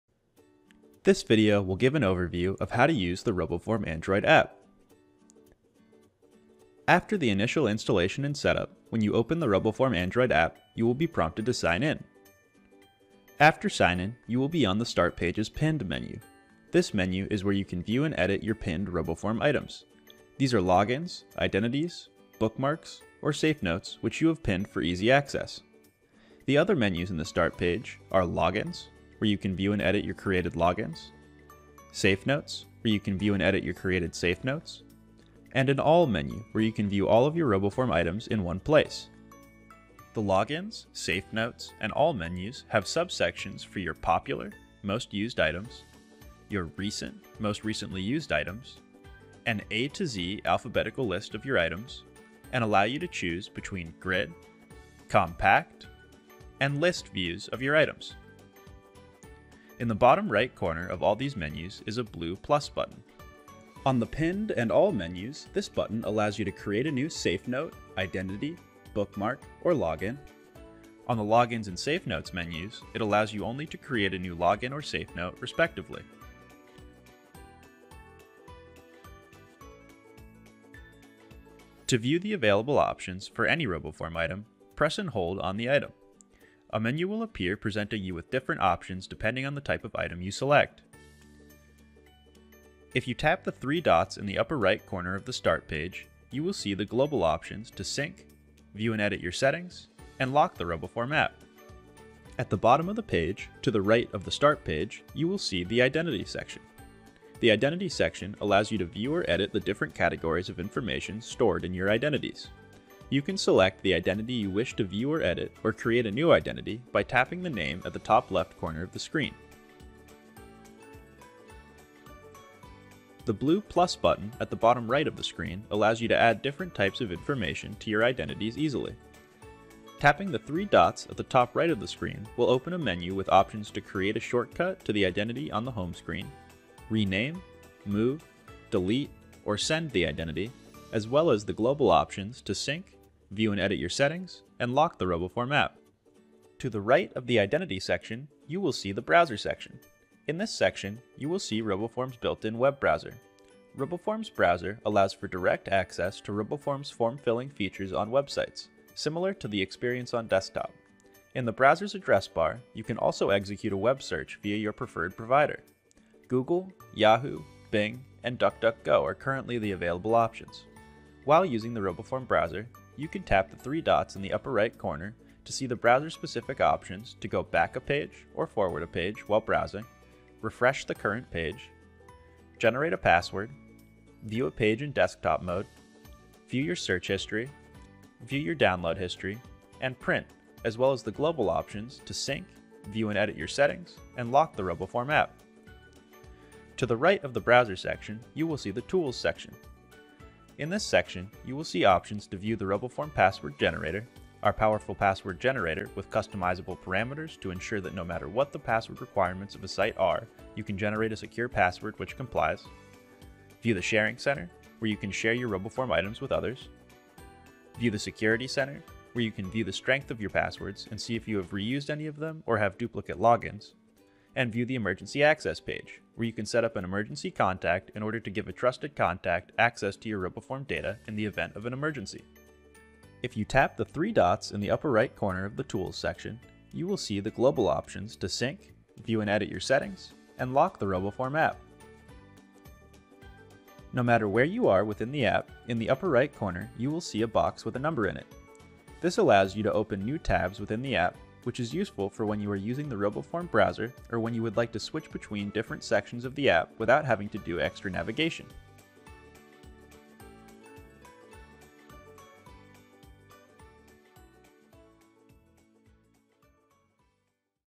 How to Use the RoboForm Android App
This video gives an overview of how to use the RoboForm Android App.
Timestamps:
0:00 - Intro
0:06 - Sign in
0:12 - Start Page
1:45 - Identity section
2:29 - Browser section
3:12 - Tools section
4:03 - Tabs